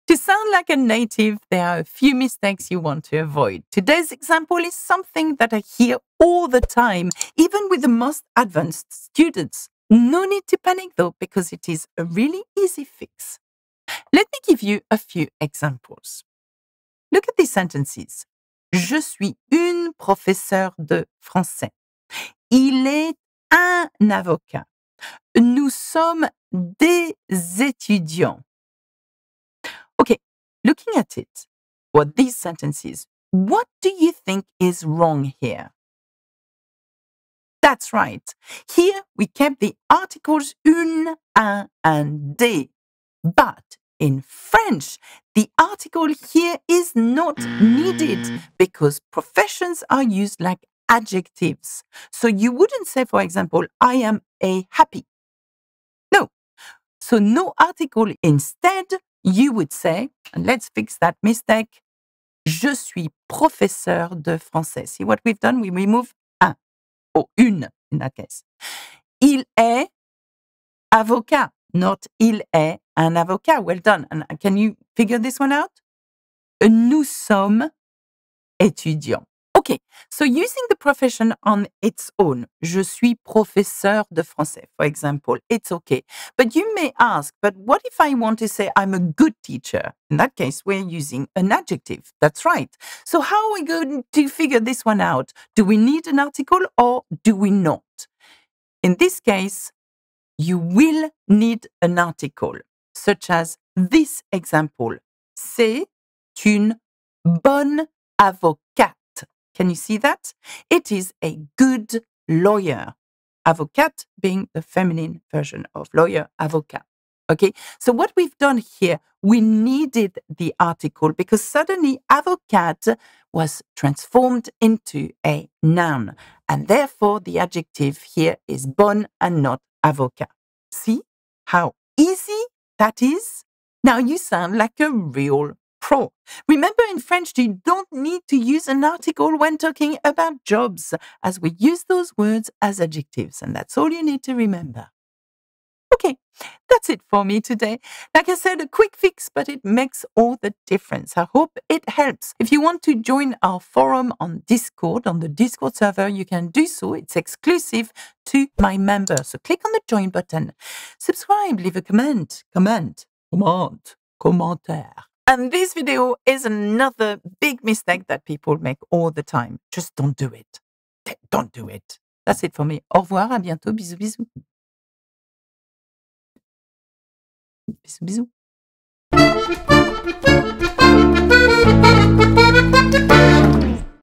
Ne dites pas 'UN, UNE, DES' ! Don't say this in french 😯
One of my top tips for sounds more French is to be accurate in your French grammar. Here is a really easy fix that will have you sounding like a native in no time.

Bisou Bisou 💋

to try out Alexa's popular French courses.